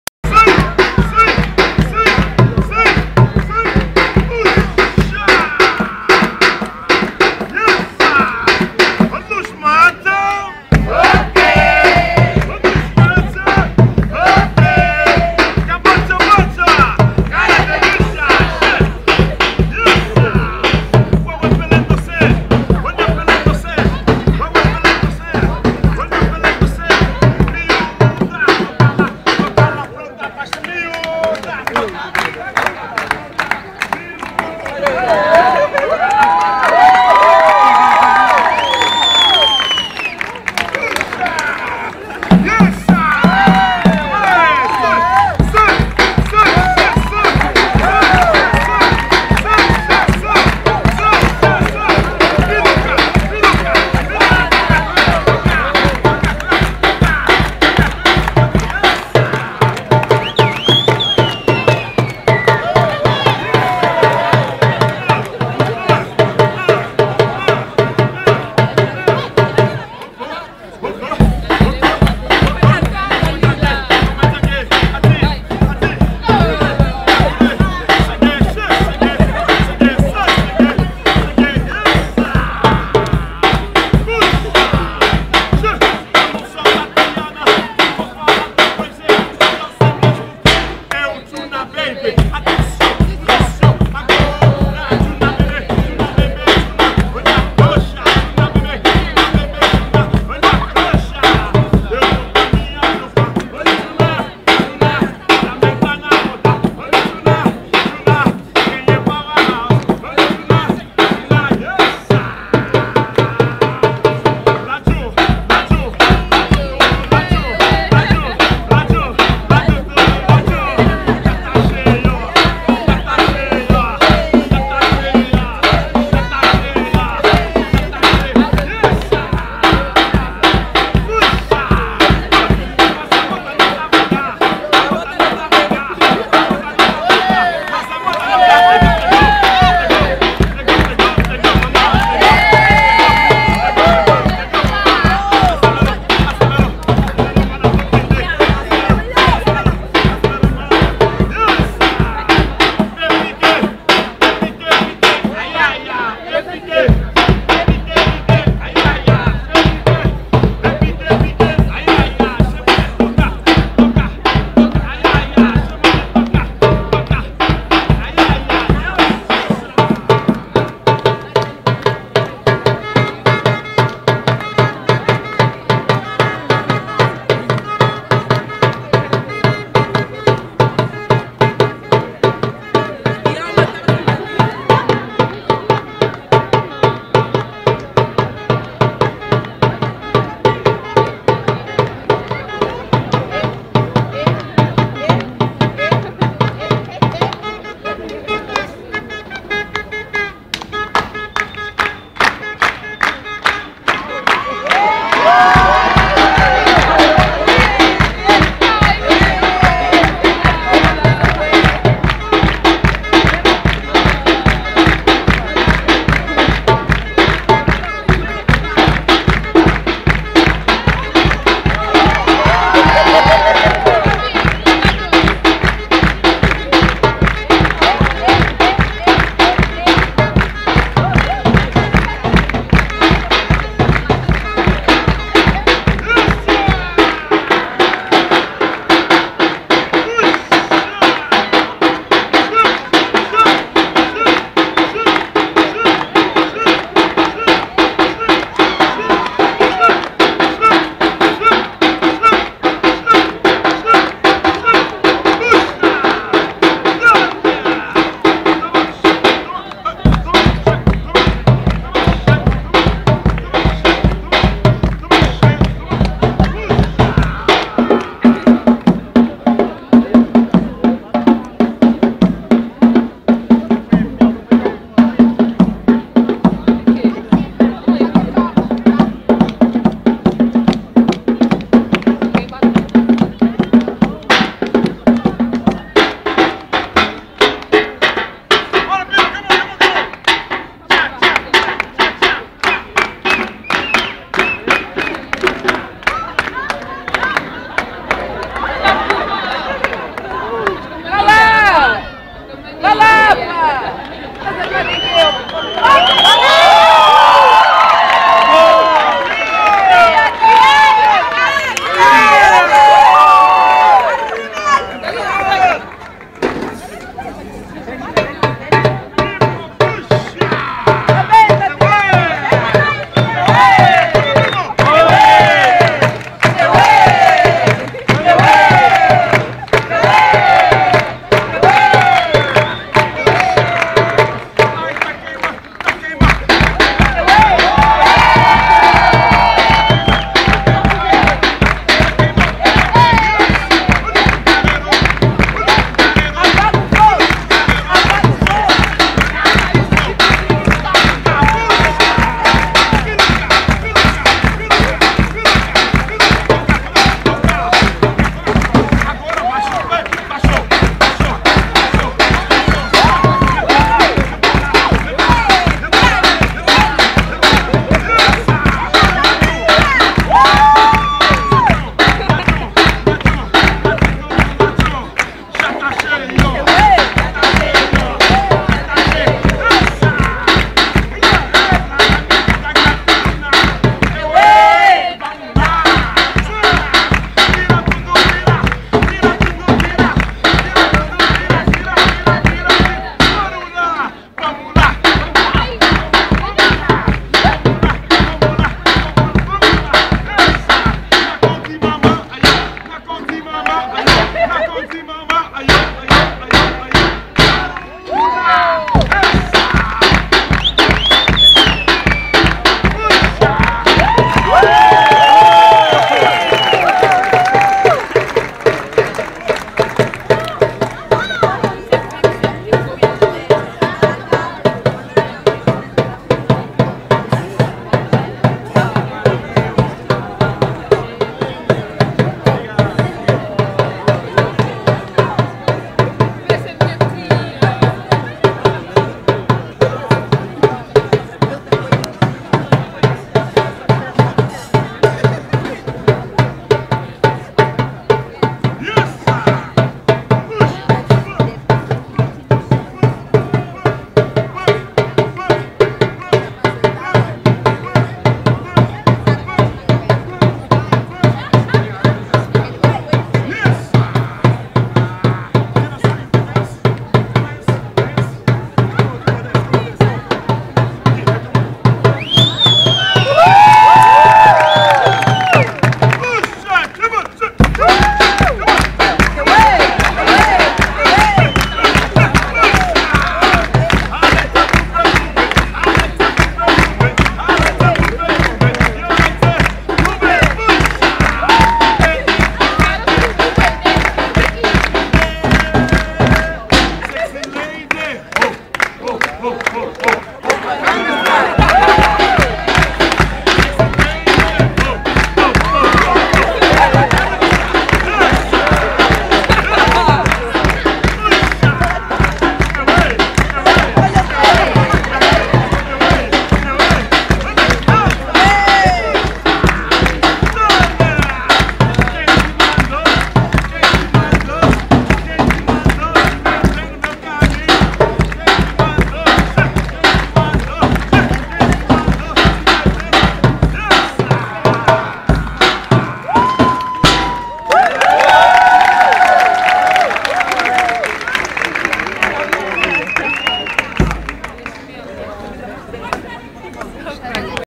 Gaspar Silva (live) - Show nas Ruas de Lisboa (Lisbon, Portugal)
Gaspar Silva is an angolan percussion player living in Lisbon.